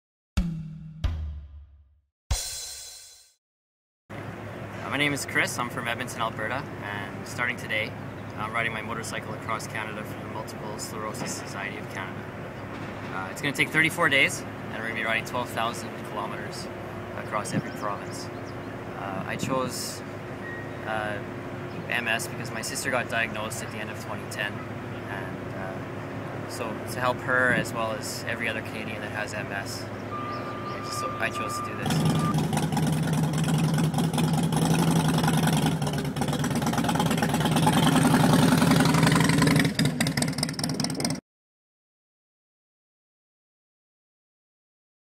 Biker raising funds for MS through cross-country ride
Tobias Romaniuk from The Telegram headed to 1 Water Street July 17 for this story.
Motorcyclist Chris Anderson is raising money for MS and riding his Harley Davidson bike on a 12,000 kilometre journey across the country. He left from the mile zero marker of the Terry Fox monument on Water Street today. The journey is expected to take just over a month. a website Anderson set-up for his fundraising effort.